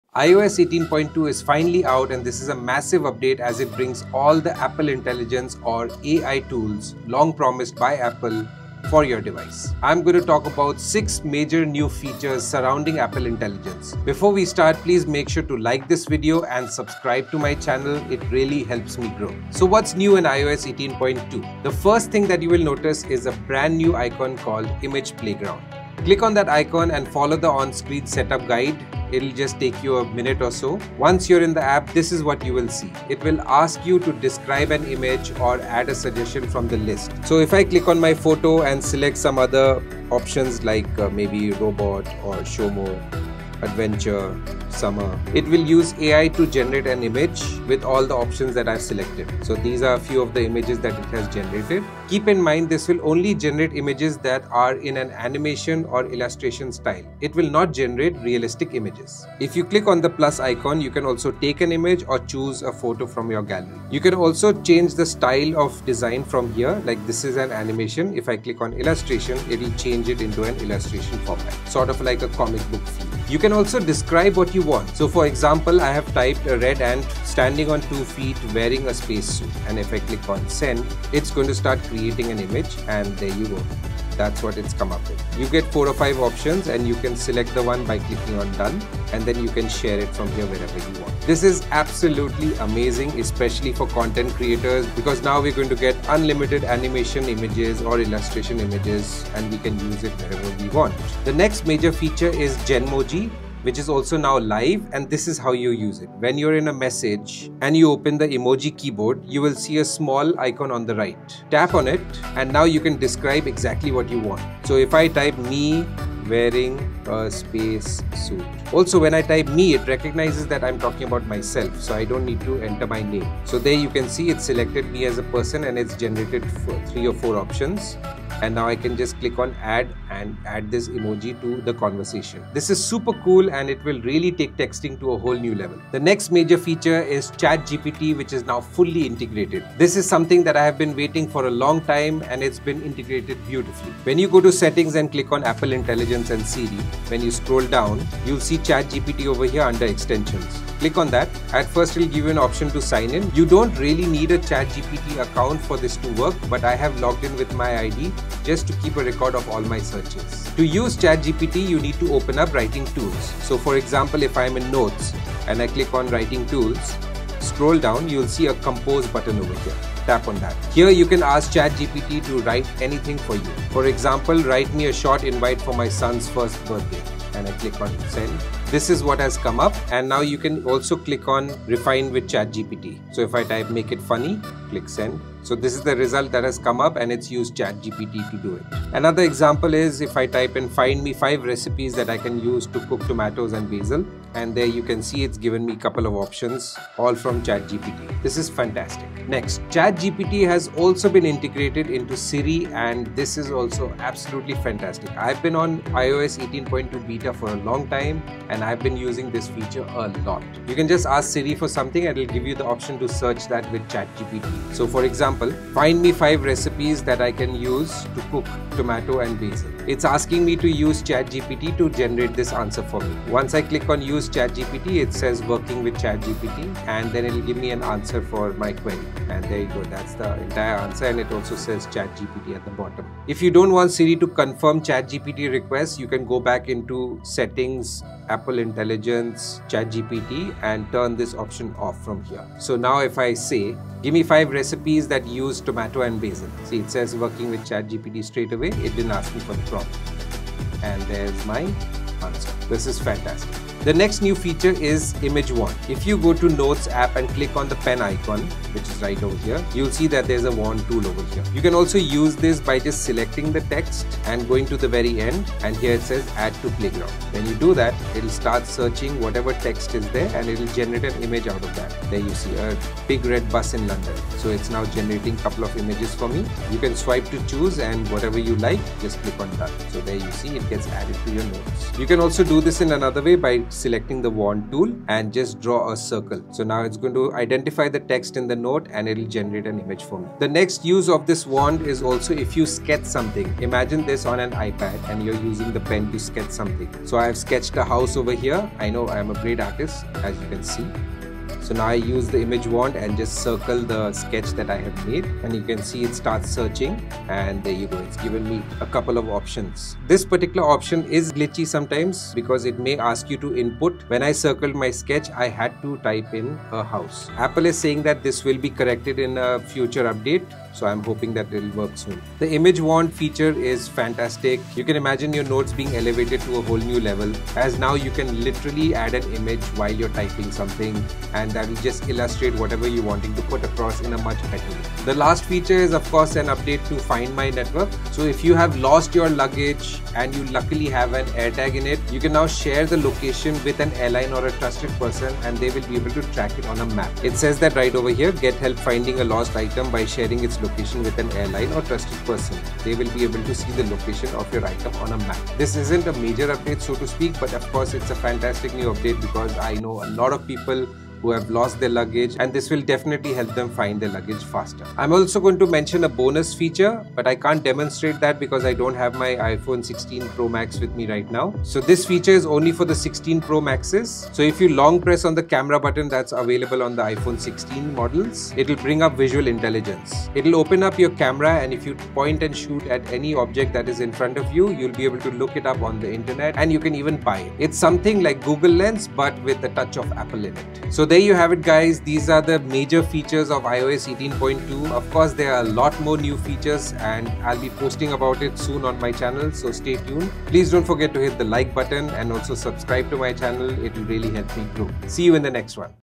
iOS 18.2 is Out : 6 Major Apple Intelligence Features!
In this video, I dive into the latest updates in iOS 18.2, focusing on six major features that showcase Apple's push toward smarter, more intuitive user experiences powered by Apple Intelligence. From enhanced Siri capabilities to improved machine learning tools, iOS 18.2 brings exciting changes to how your iPhone interacts with you. Join me as I explore these new features and how they can help you get more out of your device.

My smartphone
My tablet
My Smart Watch

DJI Wireless Mic 2
Key Light

Chapters

0:00 - Intro
0:21 - Image Playground
1:41 - Genmoji
2:15 - ChatGPT Integration
3:19 - Siri & ChatGPT
4:13 - Image Wand
5:34 - Find My Update
6:04 - Bonus Ai Feature
2:30 - Outro